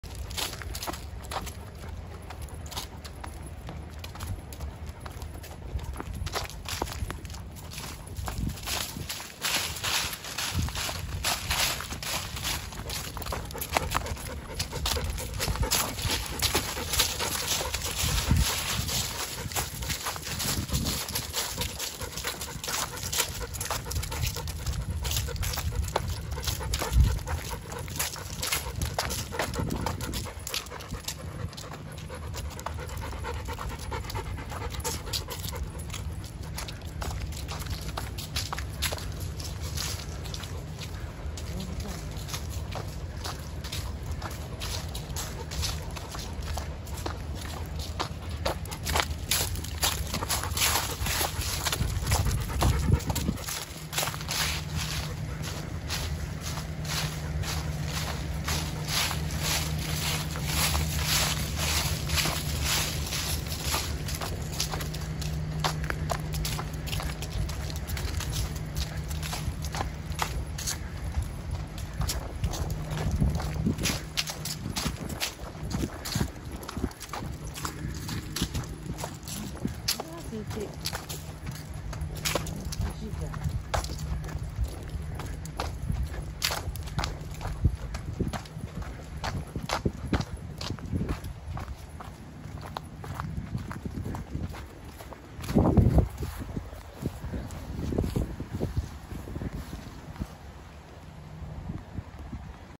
謎解き散歩 2021.12.11.
久しぶりの散歩で、アニーさんは5キロも歩きました。
さて、今回の動画でアニーさんが散歩しているところはどこでしょう？
わかる方にはわかると思いますが････
わかった方はコメントくださいね。